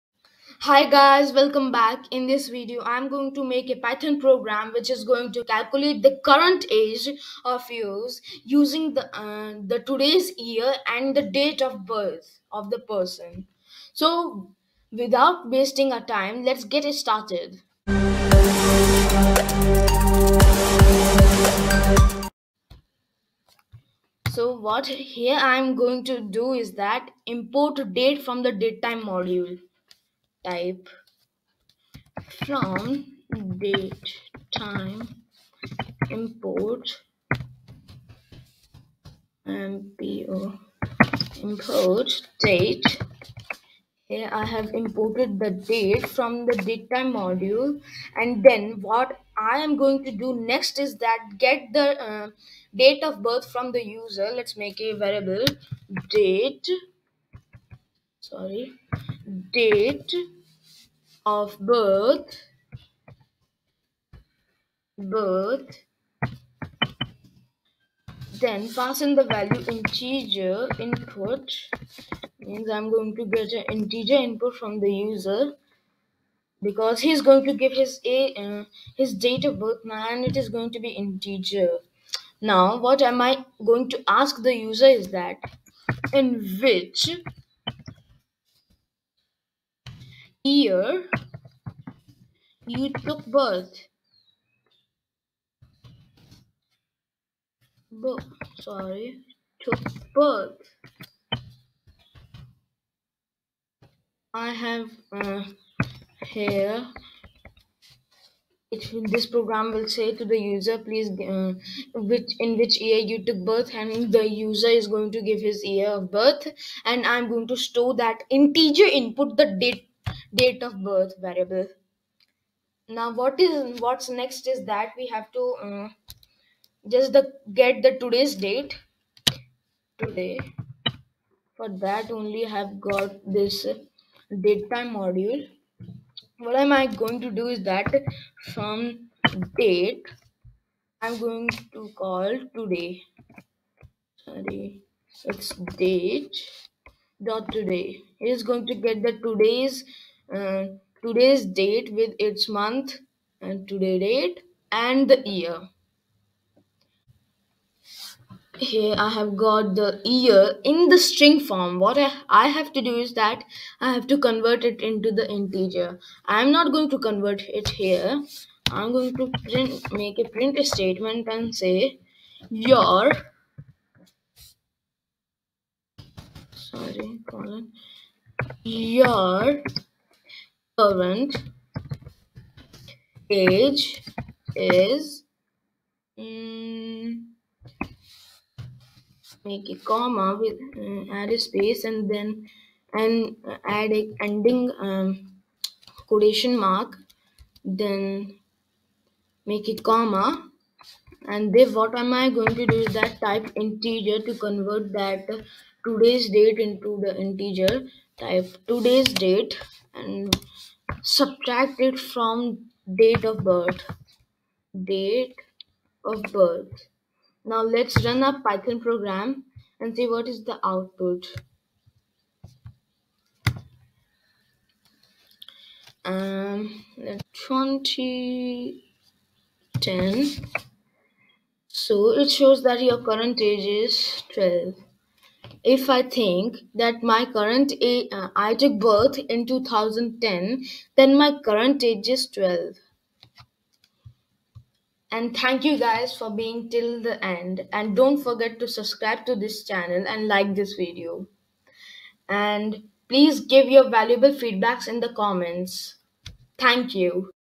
python program to calculate age using date of birth
Hi guys,
in this video, I am going to make a python program to calculate your current age only by using the date of birth and the whole source code of programs on GitHub